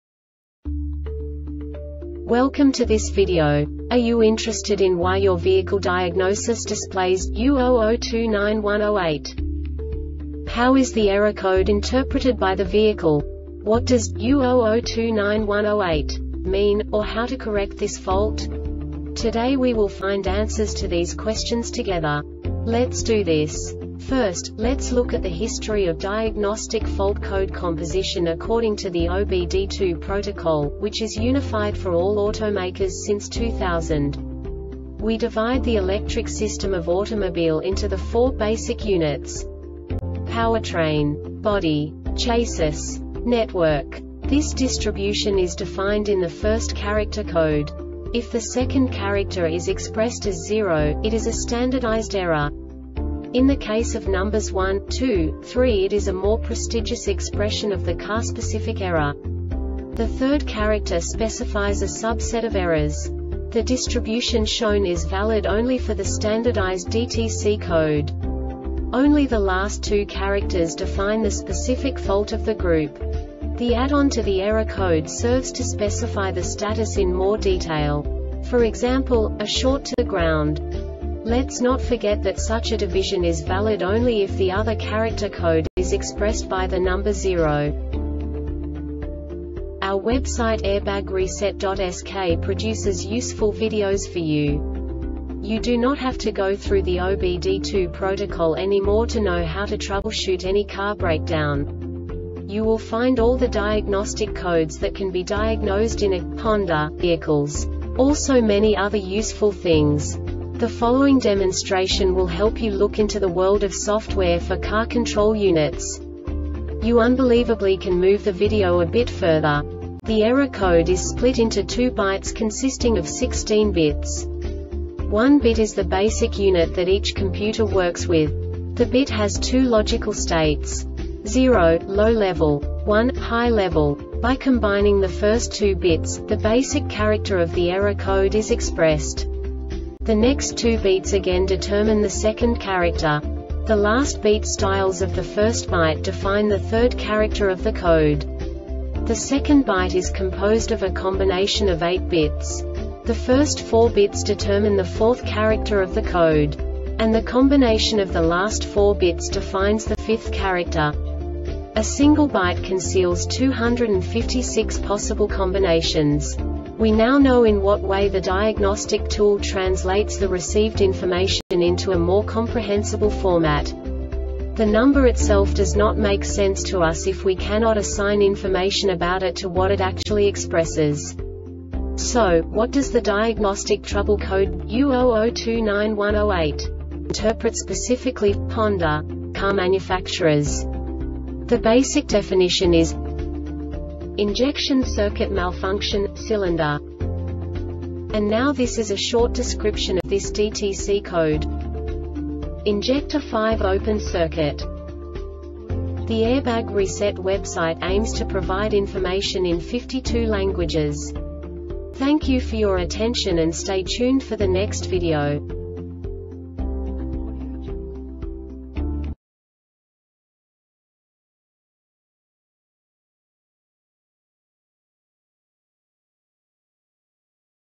DTC Honda U0029-108 Short Explanation
Contents:
0:21 Basic DTC analysis according to OBD2 protocol standard.
1:48 Insight into programming
2:58 A diagnostically understandable description of the Diagnostic Trouble Code
3:05 Basic error definition

Make:
Honda
Code:
U0029 108
Definition:
Injection circuit malfunction - cylinder 5
Description:
Injector 5 open circuit